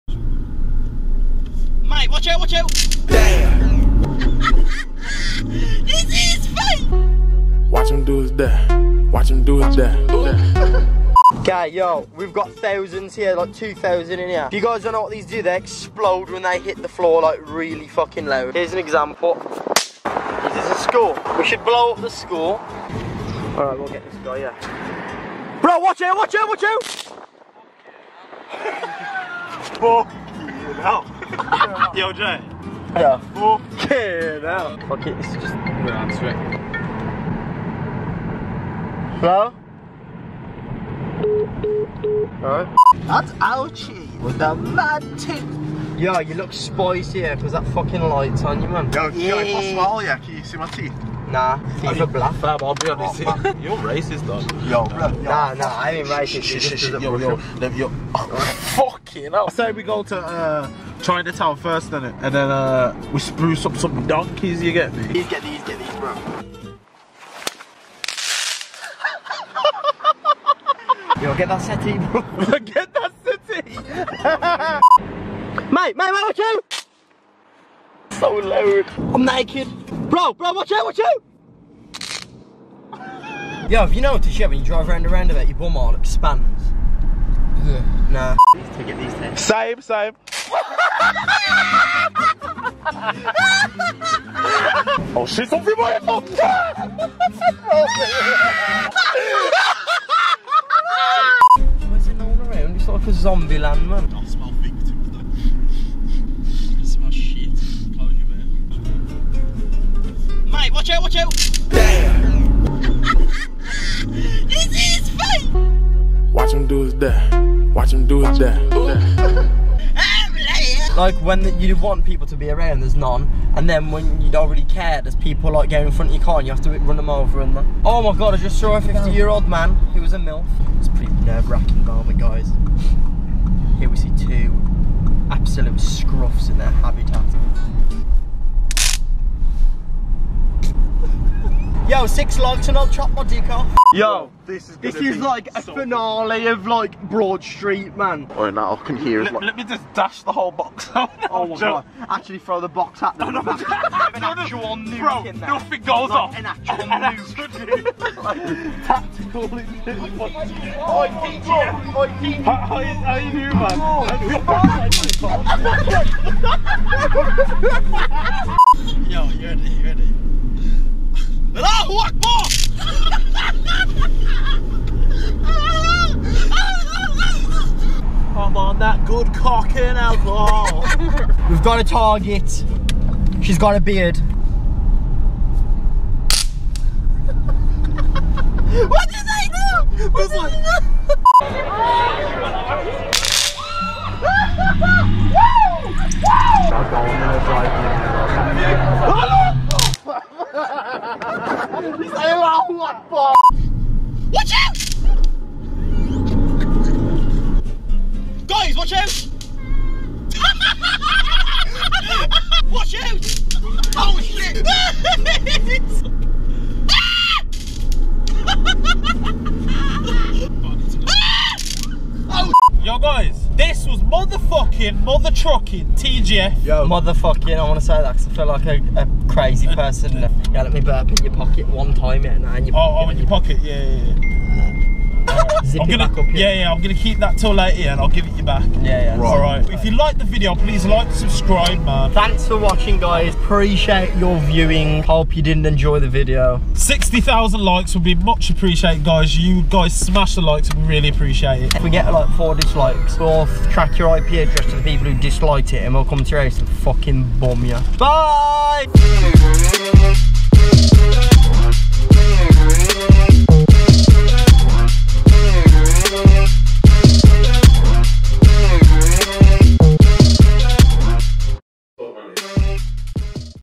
FIRECRACKER DRIVE-BY'S
Who's seen our previous WATER GUN DRIVE-BY'S and our AIR HORN DRIVE-BY'S? We attempted to do it with fire crackers this time! Love to see the comments from you guys on this one!

Romells Snapchat - pickledcookie